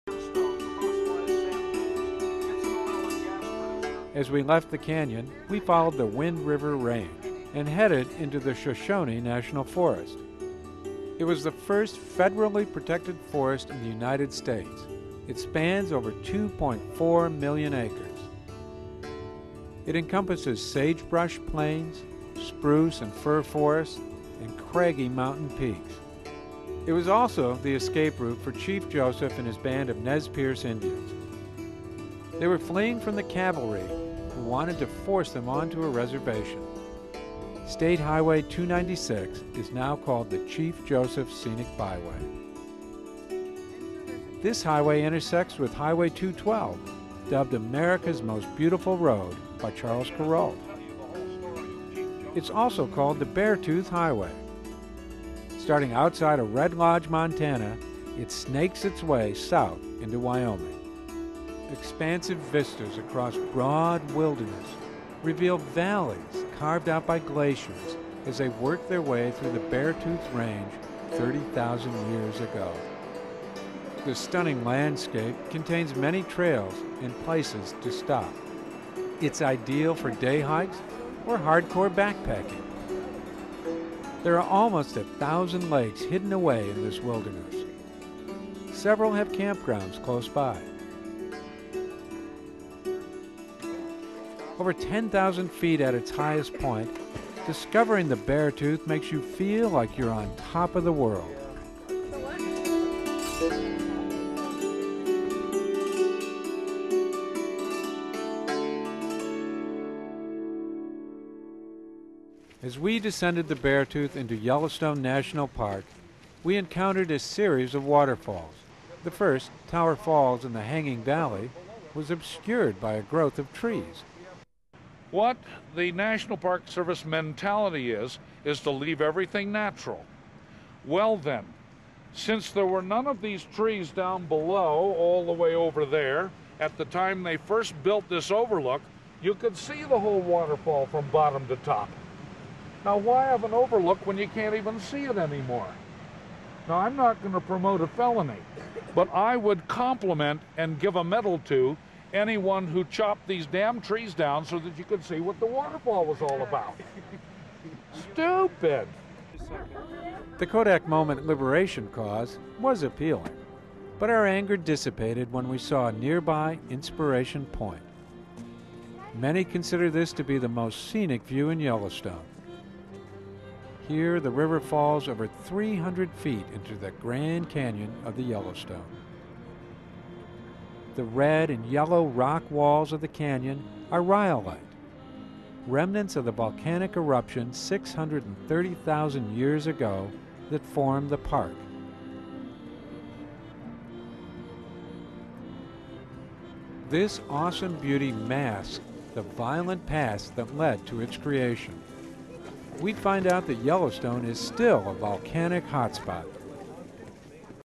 Beartooth Hwy -- Most Beautiful Road in America
A drive over the fabled Beartooth Highway through Wyoming, into Montana and back down into Yellowstone.